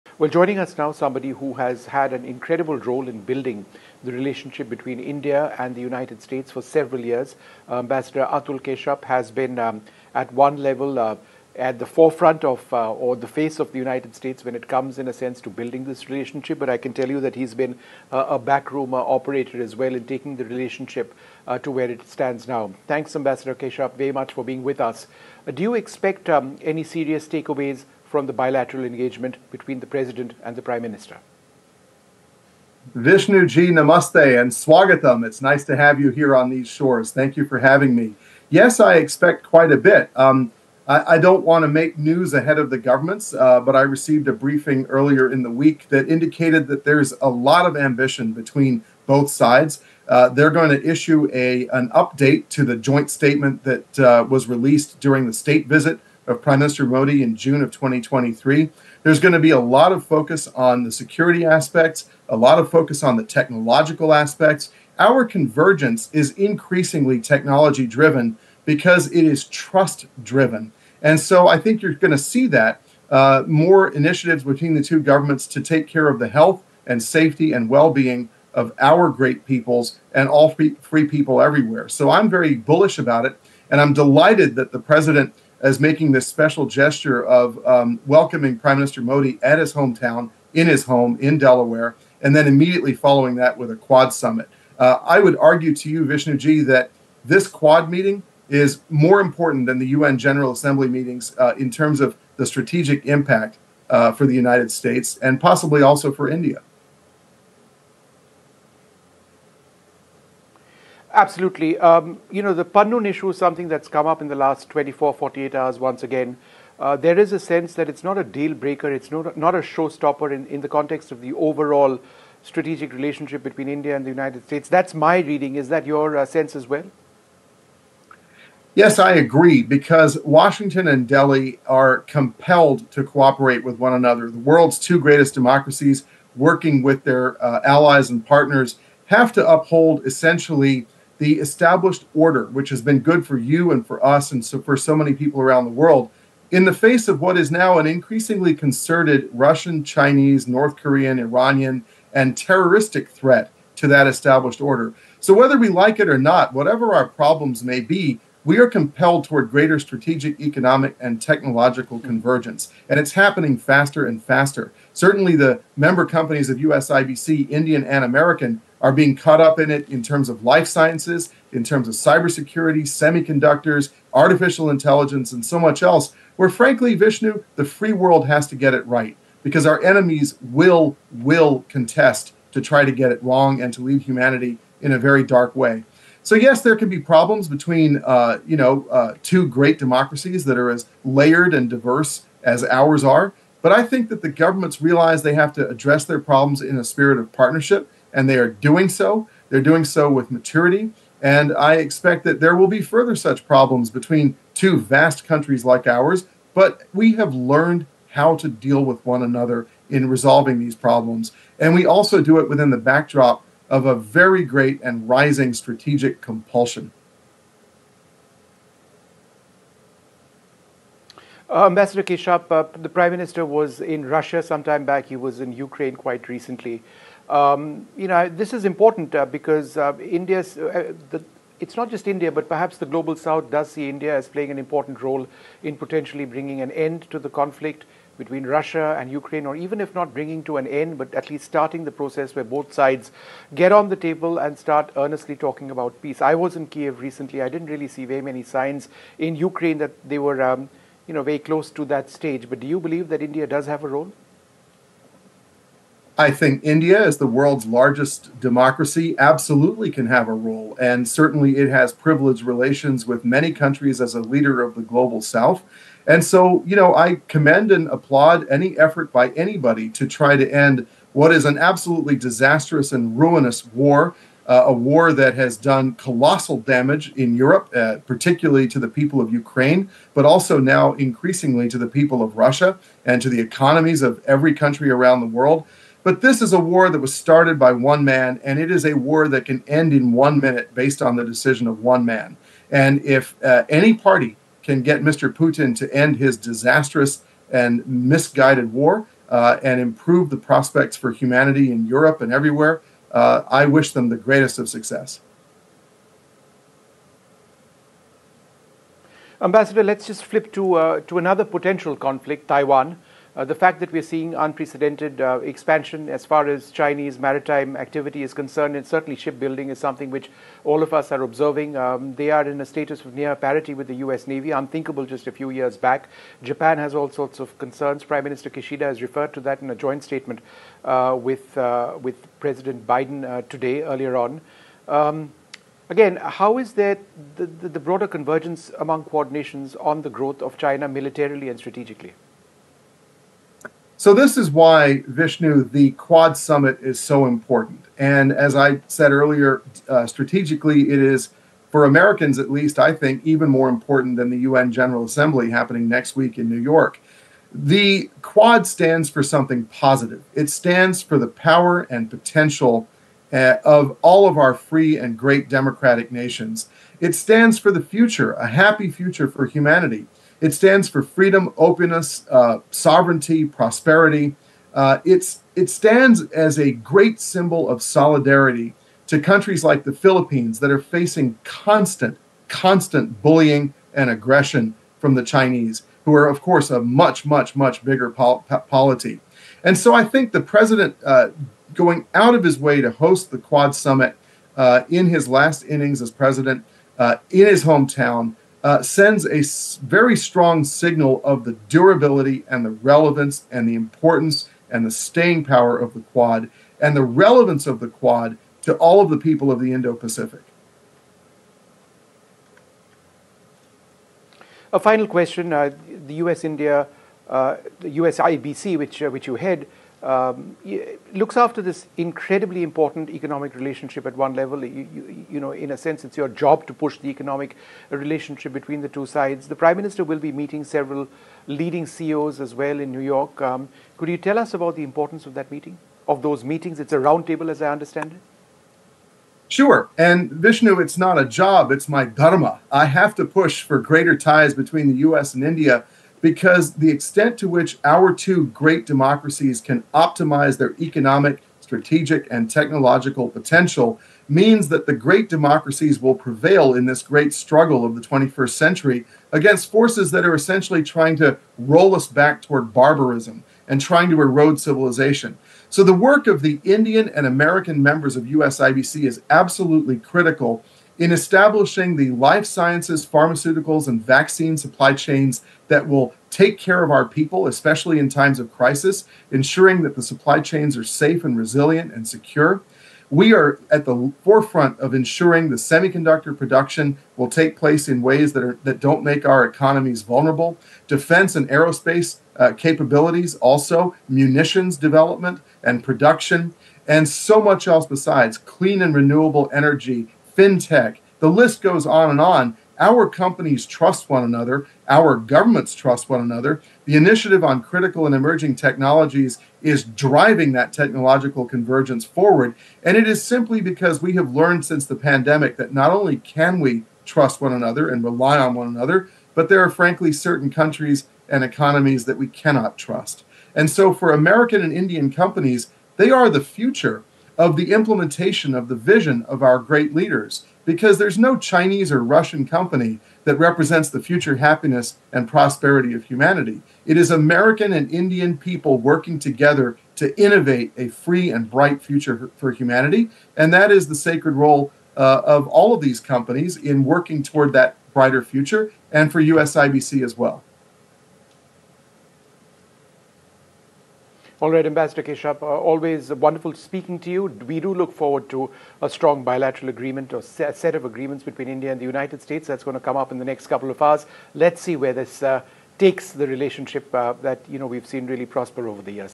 PM Modi Visit To US | For US, Quad Summit More Important Than UN Meetings: India's Ex-Envoy To US
The QUAD meeting, set to be attended by Prime Minister Narendra Modi on Saturday, is more important than the UN General Assembly meetings in terms of strategic impact for the US, and also for India, said former Indian envoy To US Atul Keshyap.